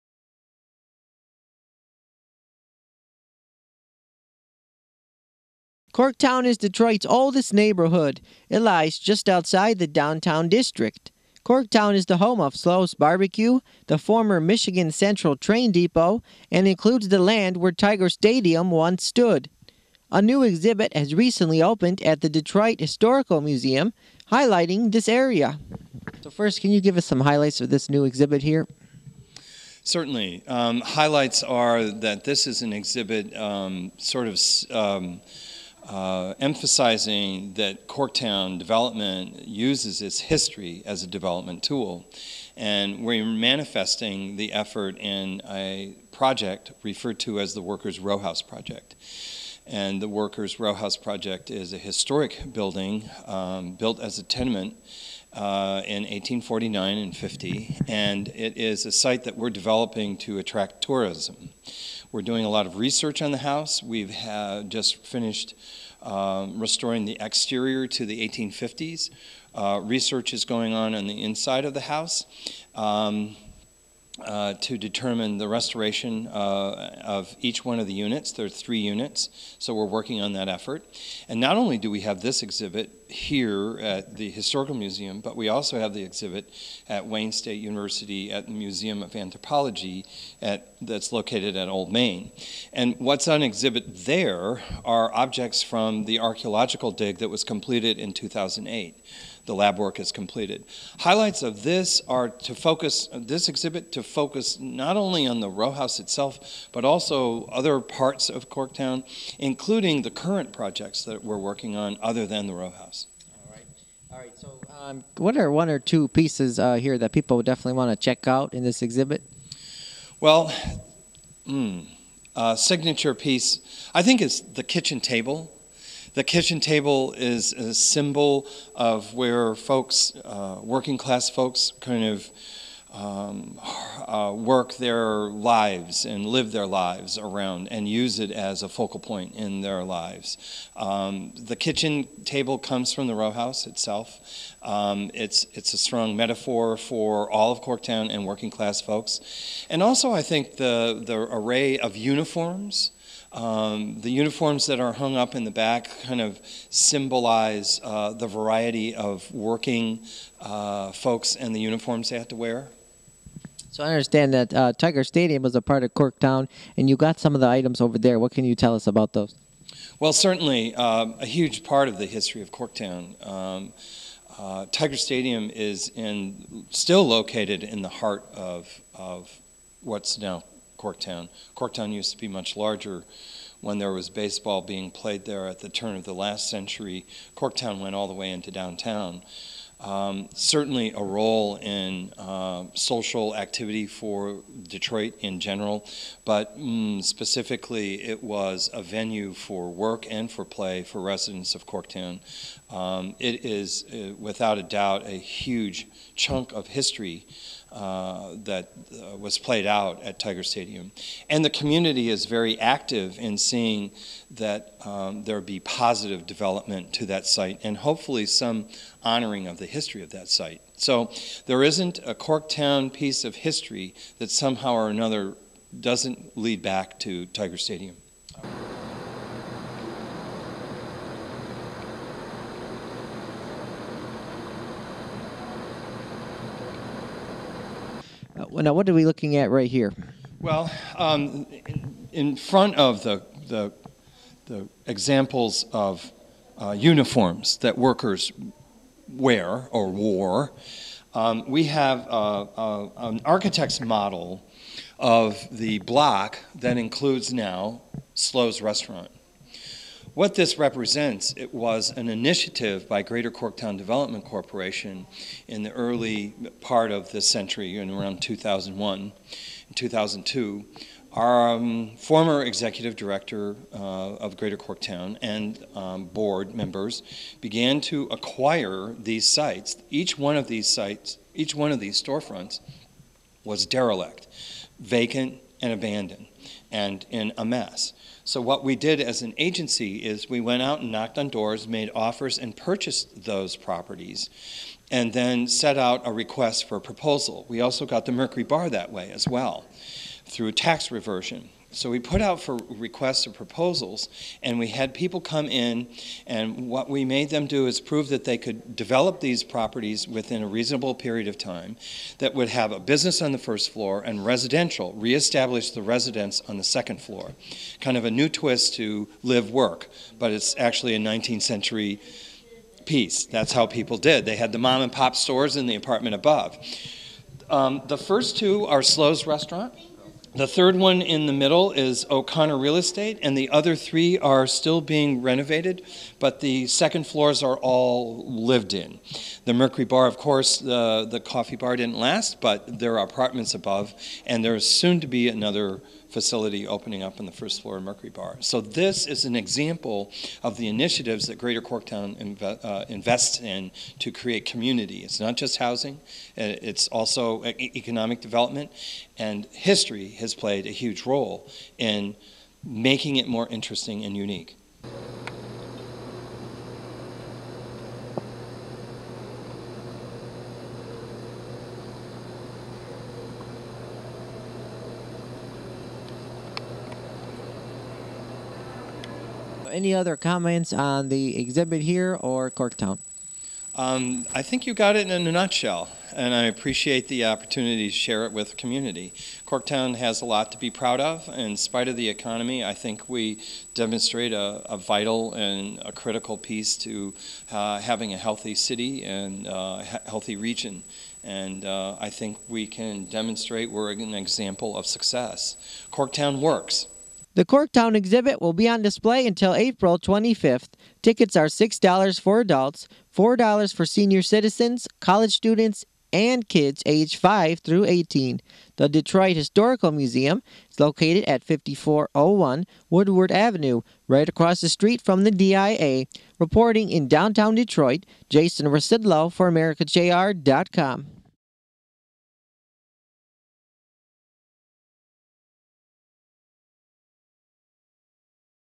New Corktown Exhibit Opens at the Detroit Historical Museum
AmericaJR.com's Jason Rzucidlo spoke one-on-one with Timothy McKay, Executive Director of the Greater Corktown Development Corporation at the Detroit Historical Museum. A new exhibit has opened at the museum titled "Corktown Works!" It features two rows of seats from the former Tiger Stadium, a photo of the old Michigan Central Train Depot, outfits worn by workers in the area, and much more. The exhibit also features the Workers Row House, which will serve as the new museum for the Corktown area when it is completed in about a year.